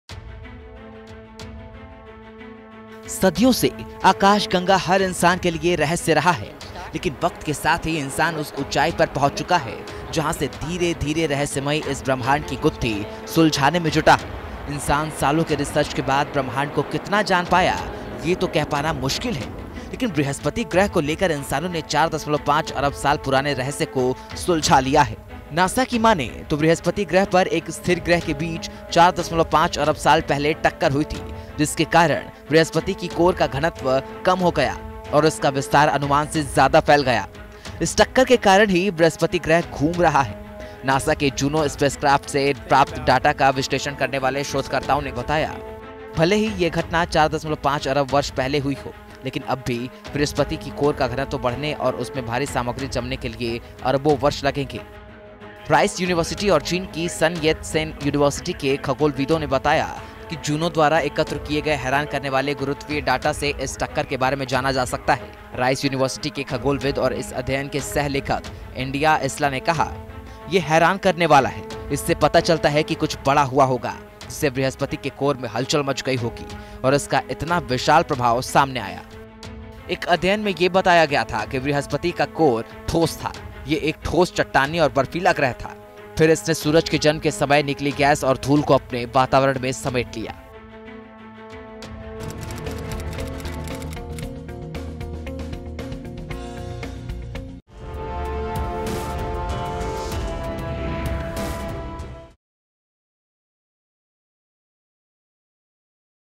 JUPITER पर NASA का 4.5 अरब साल पुराना खुलासा !
नासा के जूनो स्पेसक्राफ्ट से प्राप्त डाटा का विश्लेषण करने वाले शोधकर्ताओं का कहना है कि बृहस्पति और एक स्थिर ग्रह में 4.5 अरब साल पहले हुई टक्कर की वजह से बृहस्पति की कोर का घनत्व कम हो गया और इसका विस्तार अनुमान से ज्यादा फैल गया...

A cosmic, head-on collision between Jupiter and a protoplanet 4.5 billion years ago, could finally explain surprising readings made by NASA’s Juno spacecraft. NASA now believes this collision with the super-dense planet, can account for Jupiter’s strangely gassy inner-core.